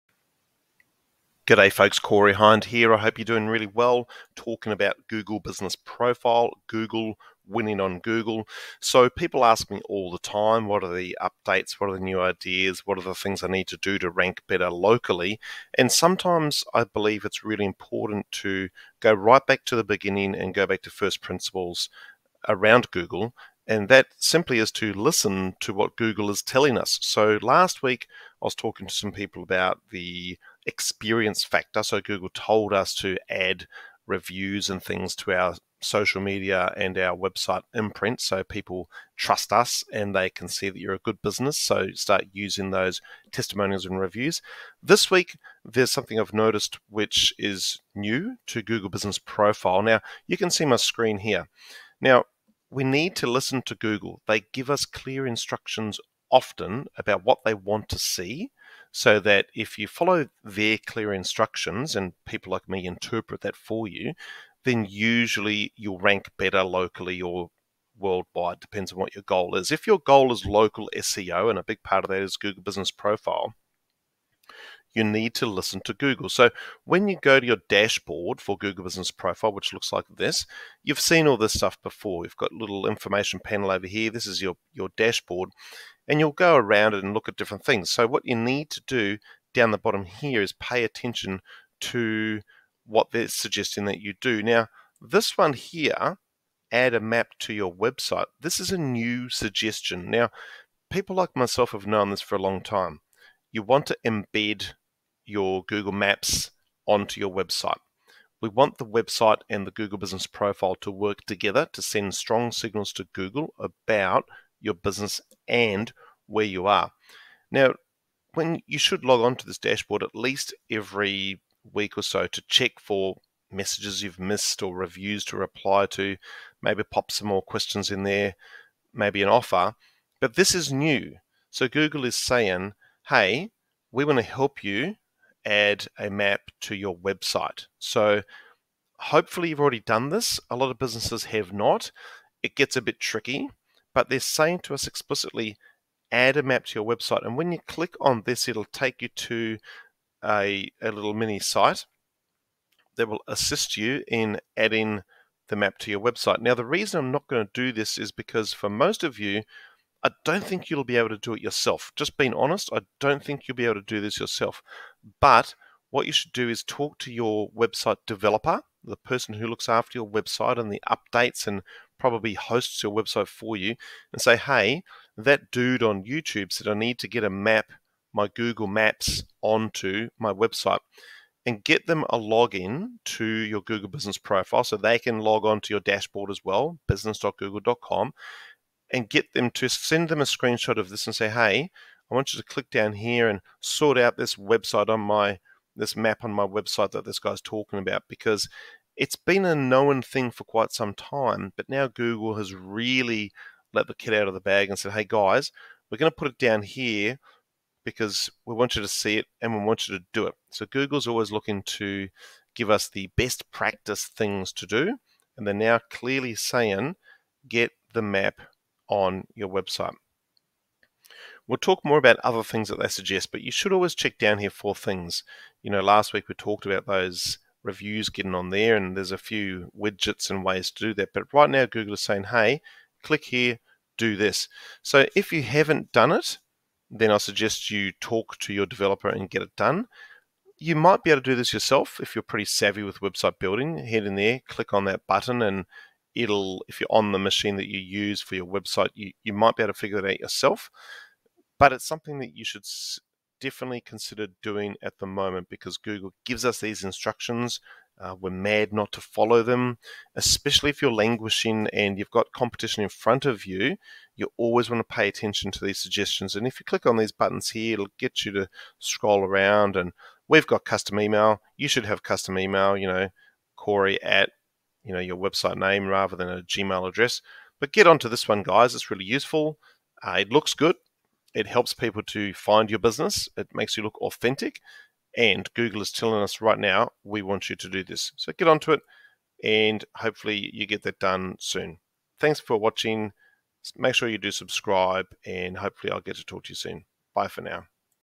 How to add your Google Business Map to your website
Your Google Business Profile dashboard gives you good tips on what to do, to dominate and win your local category for Google Search.

Having your Maps listing embedded on your website is something that you should do.  Google has added this as a suggestion to many Google Business Profile dashboards.

Watch on - then talk to your developer and get this done.